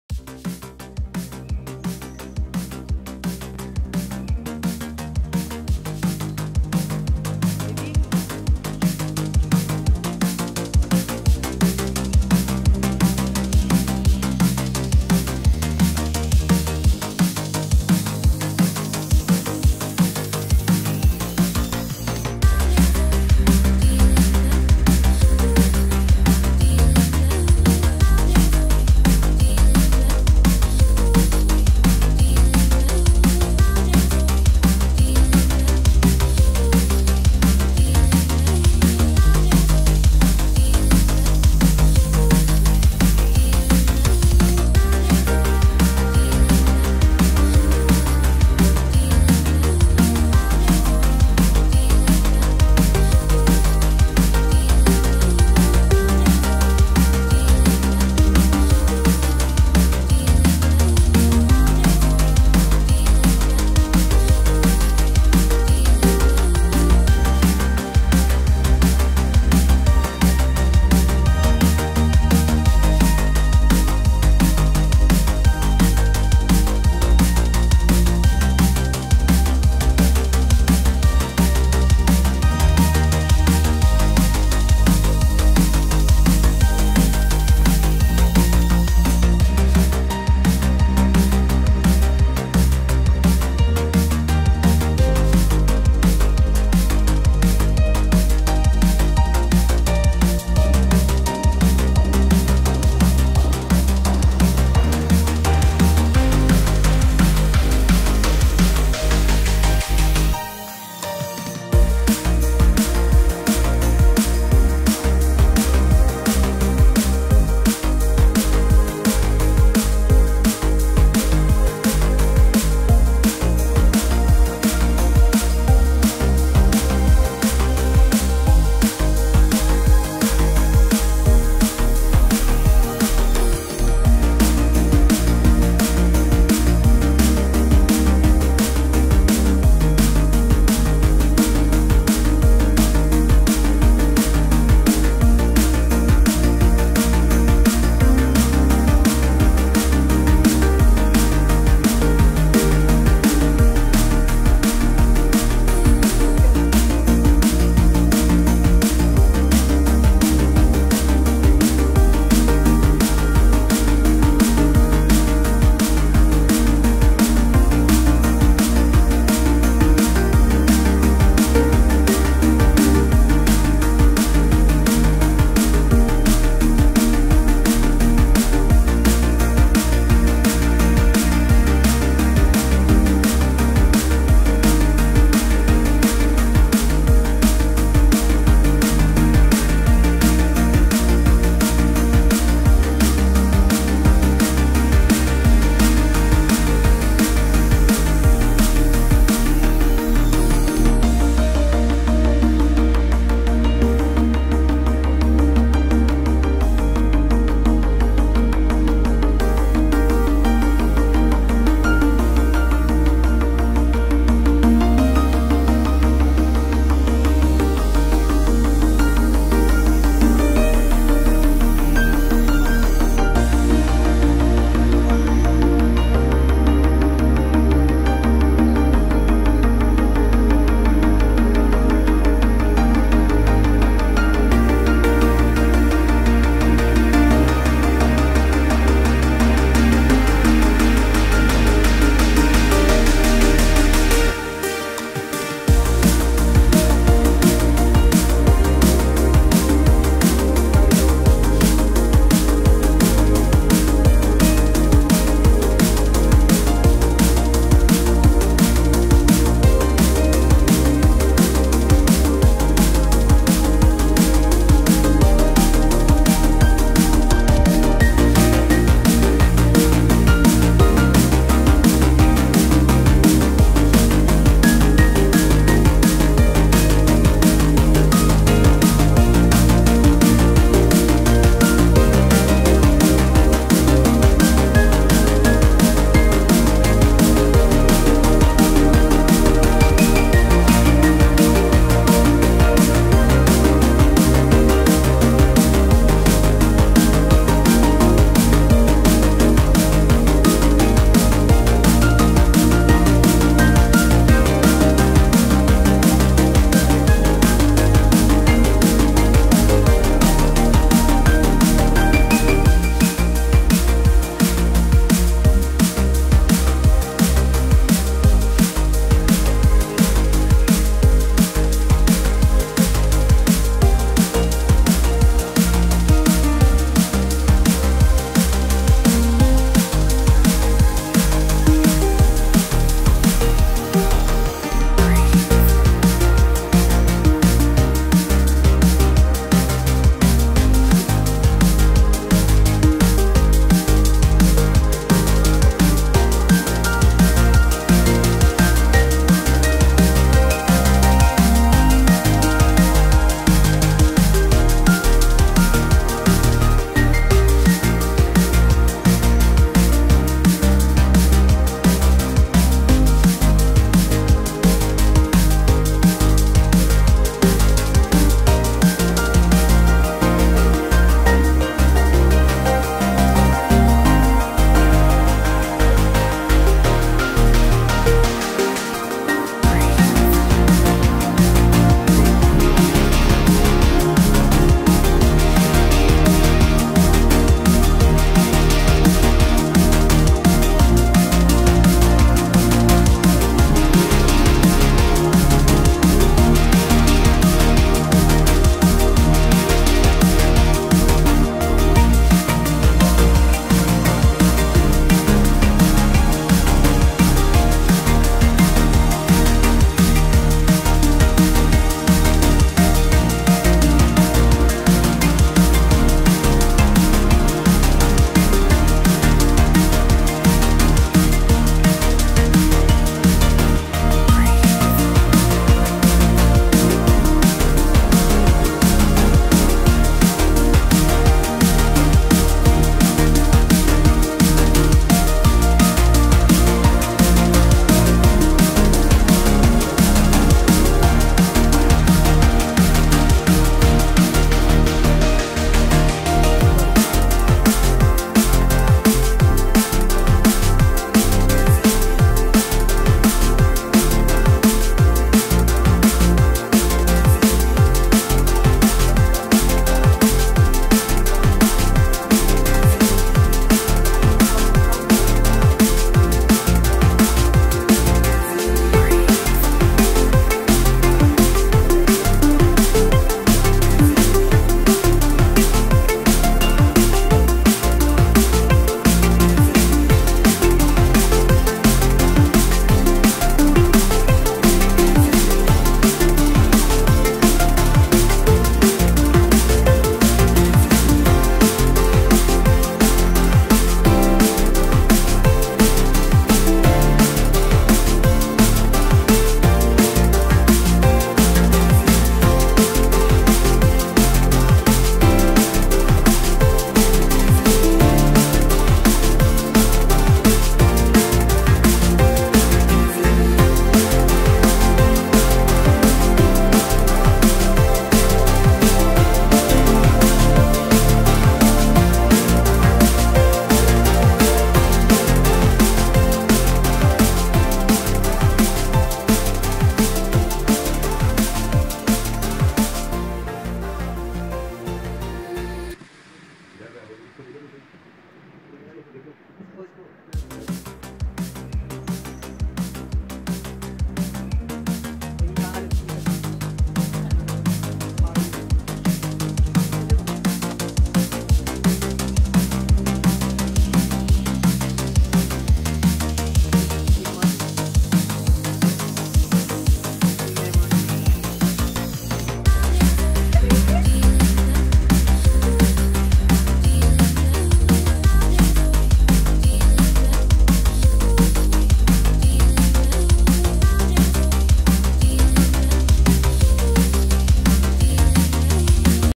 My cat go wild / cute kittens / funny animals
Playing fighting of cats and funny videos